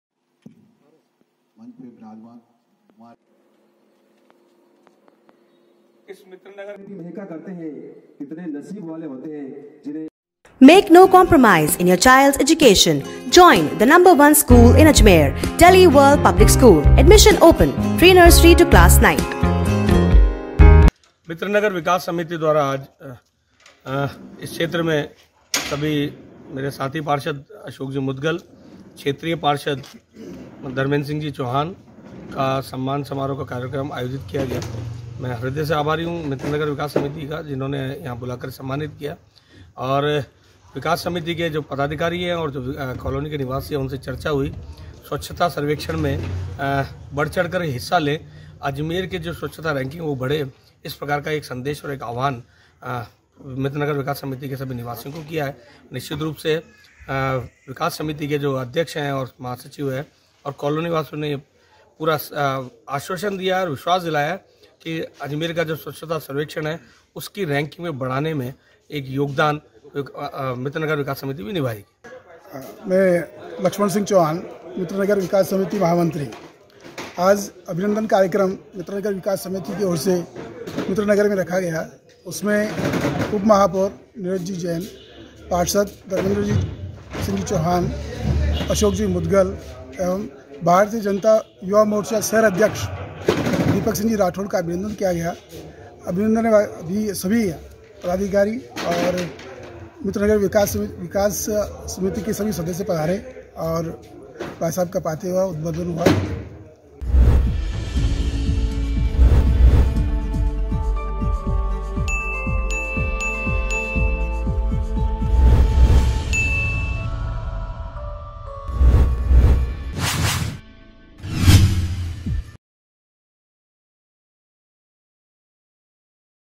Ajmer | मित्र नगर विकास समिति ने जीते हुए वार्ड पार्षदों का रखा सम्मान समारोह | MTTV INDIA
मित्र नगर विकास समिति ने जीते हुए वार्ड पार्षदों का रखा सम्मान समारोह
उप महापौर नीरज जैन सहित अन्य वार्ड पार्षद रहे मौजूद